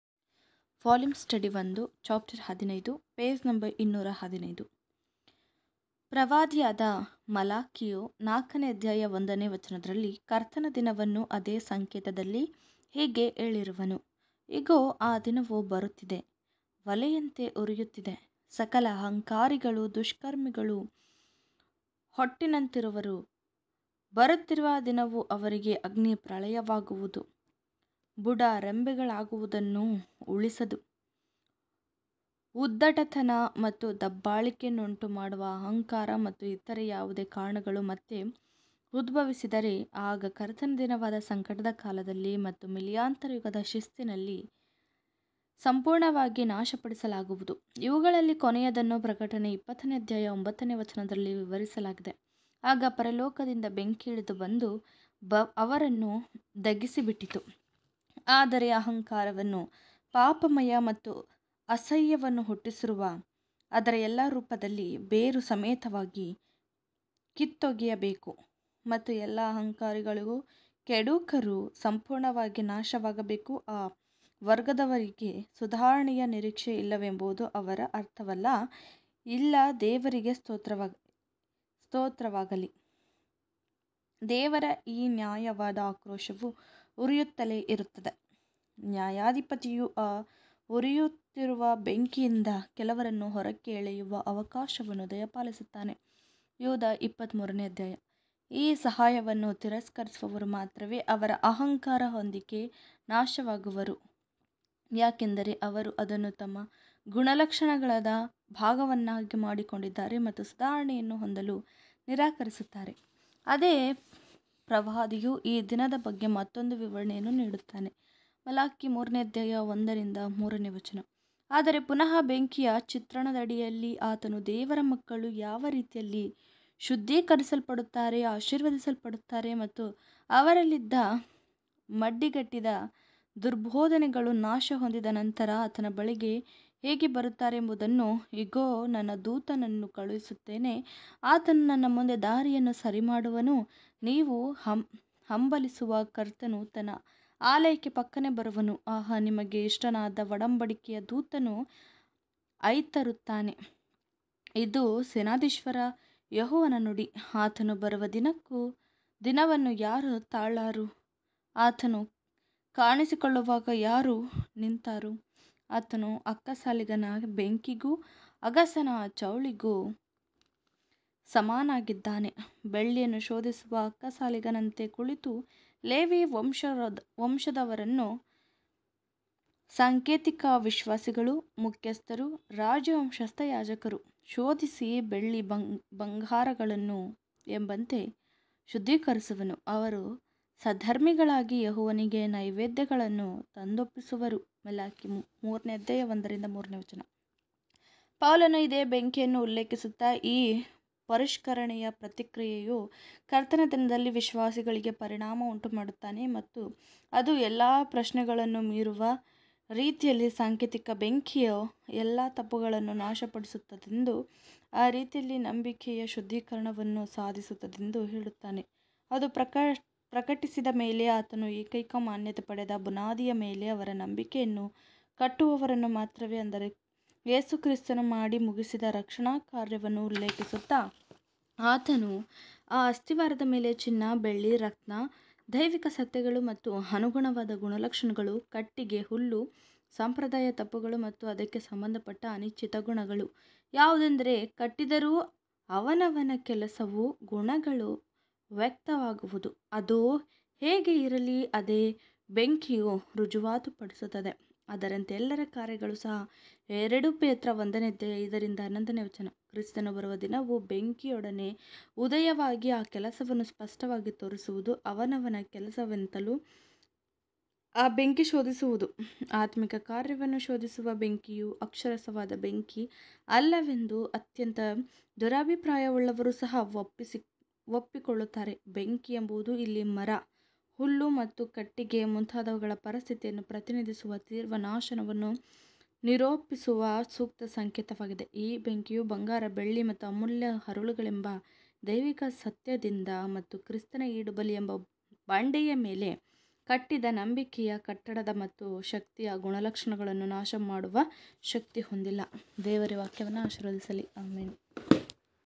🎥 Audio Recording (Kannada) 📚 1st vol 📖 Ch 15 THE DAY OF JEHOVAH 📄 Pg 215 Free Bible Study Br Raju
🎥 Audio Recording (Kannada) 📚 1st vol 📖 Ch 15 THE DAY OF JEHOVAH 📄 Pg 215 Free Bible Study Br Raju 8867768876 - Jesus Loves Ministry